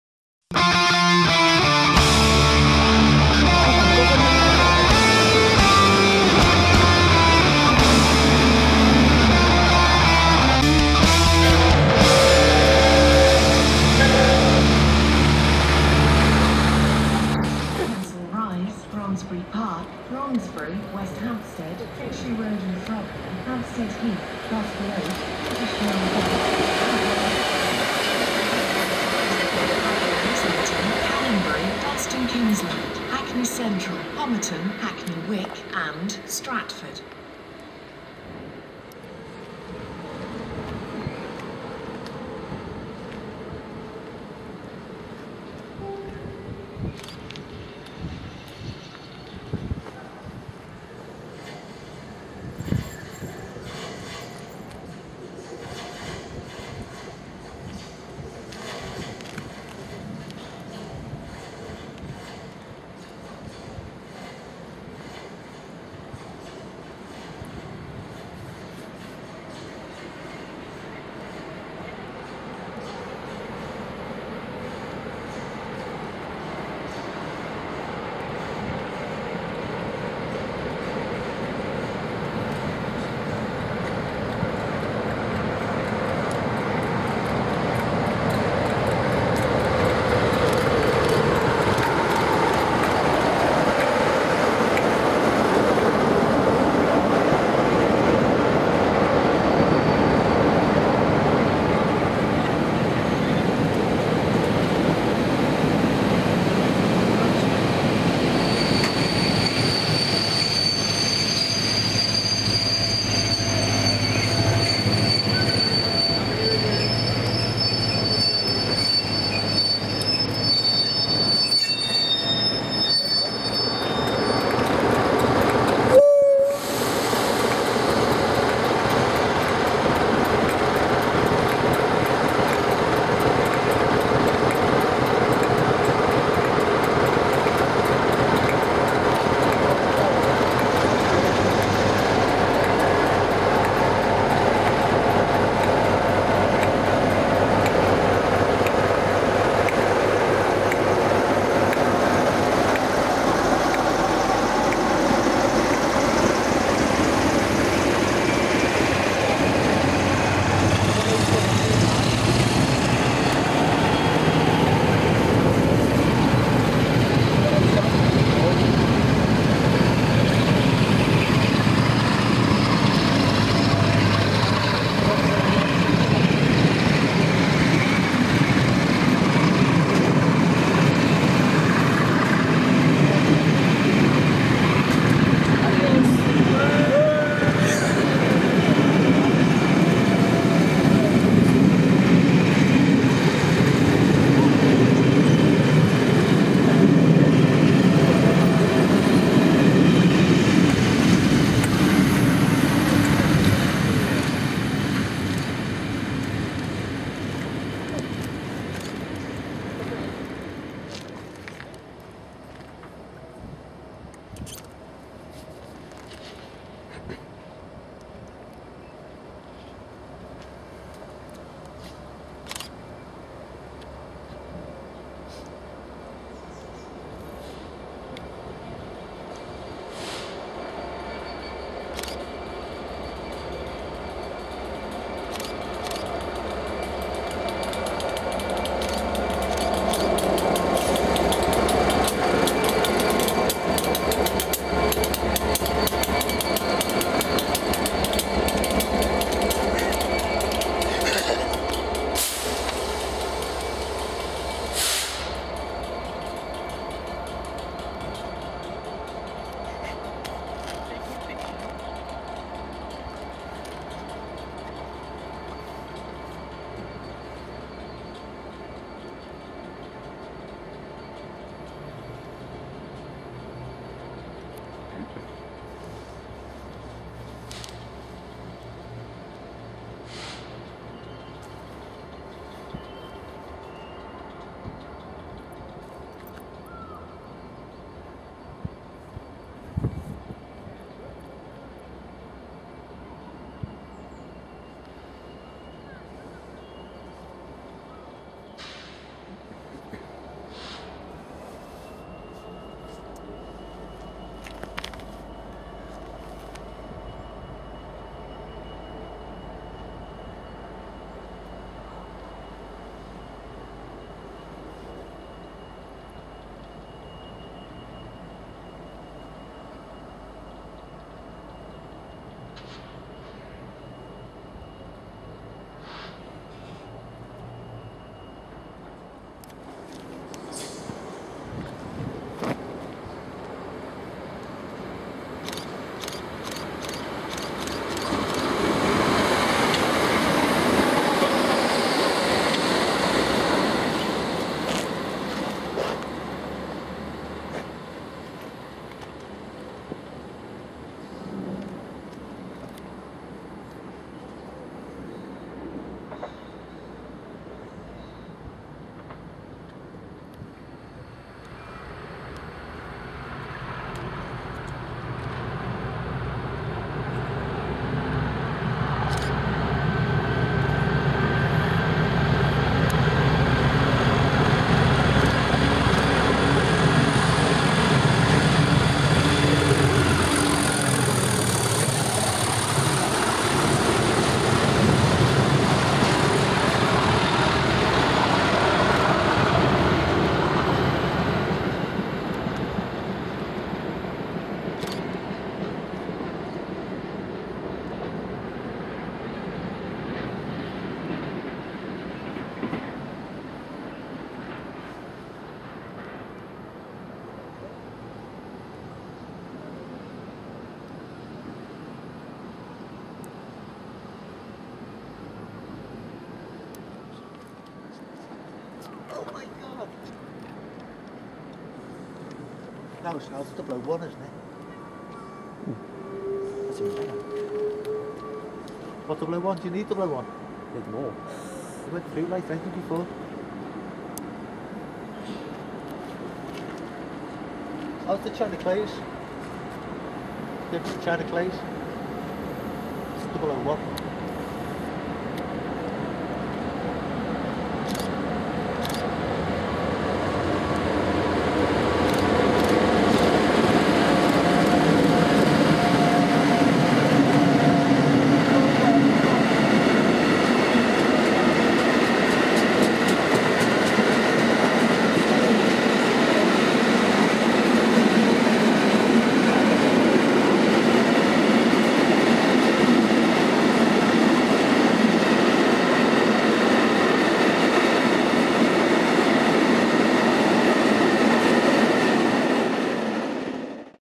Variety at Kensington including tractor and spoon thrash
A few hours at Kensington Olympia brought some interesting results. 66132 and 66207 pass working 0Y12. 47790 in its new livery pulls in with the Northern Belle ECS with 47810 at the other end, and departs several minutes later with thrash clag and a horn, working 1Z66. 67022 is the next to pass working 0M05, followed in the opposite direction by 319449 working to Cricklewood (?). This is followed by 37069 and 37423 thrashing on full power with 6M95. The final sighting is 92001 working south with 6B63.